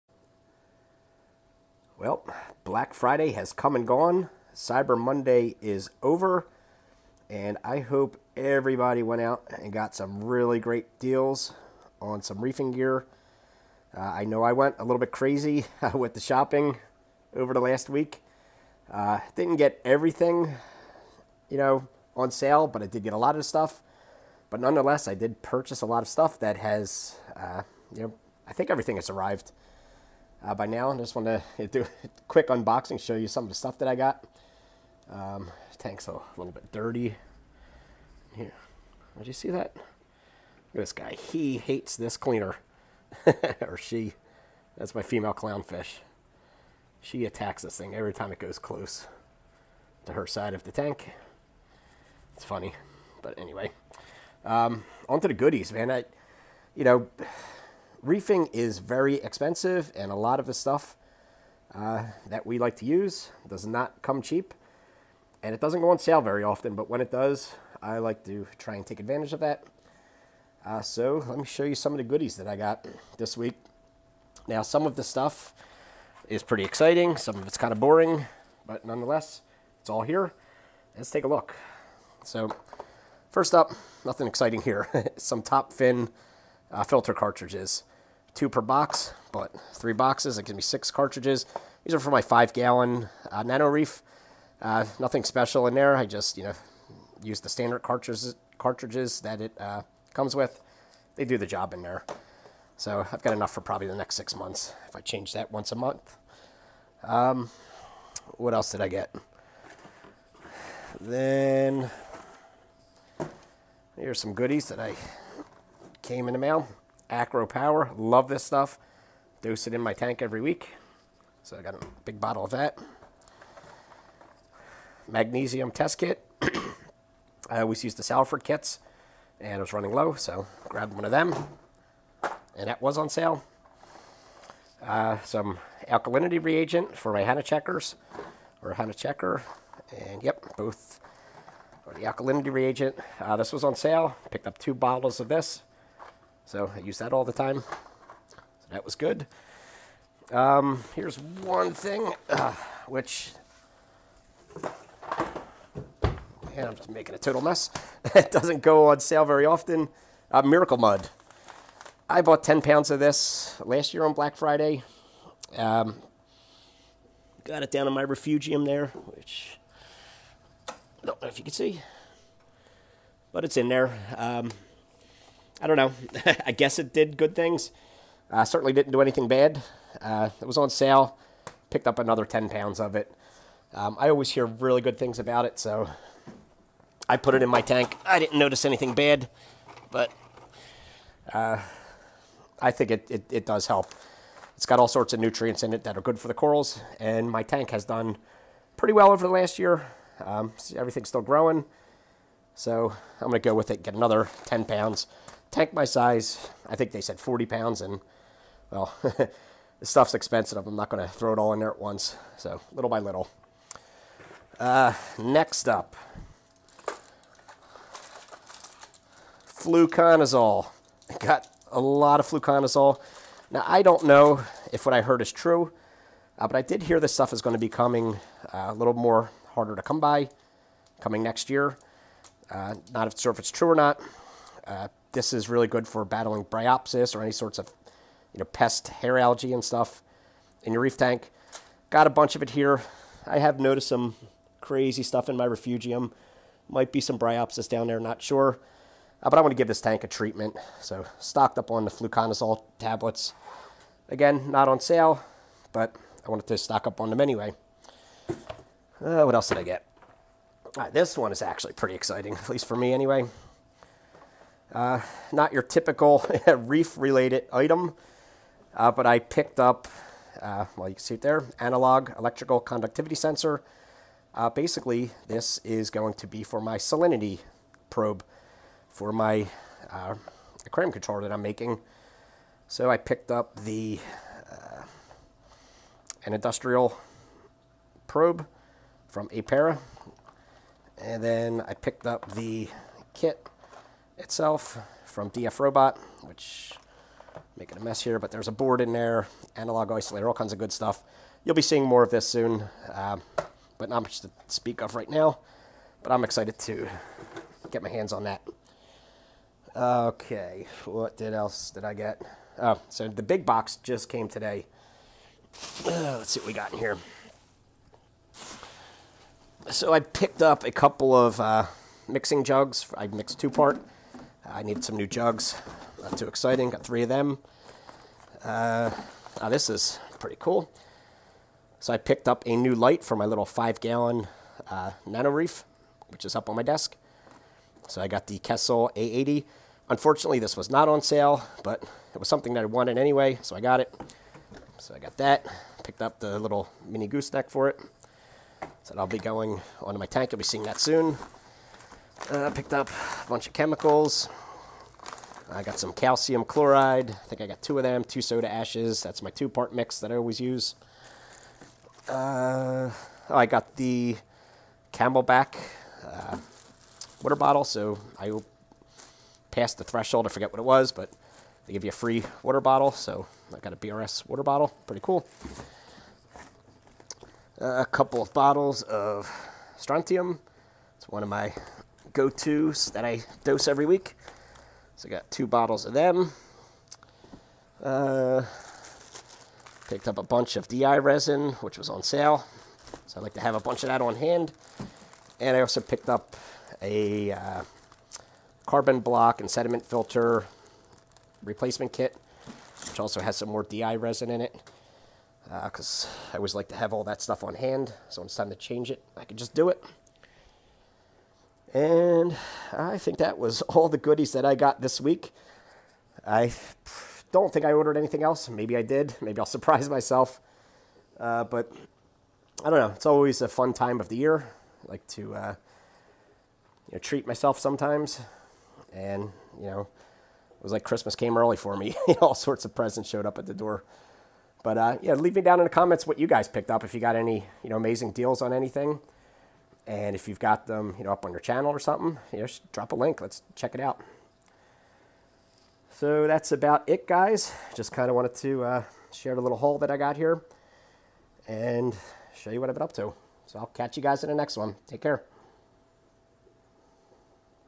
Black Friday Reefing Haul
I got lots of goodies during Black Friday, come check out what's in the box!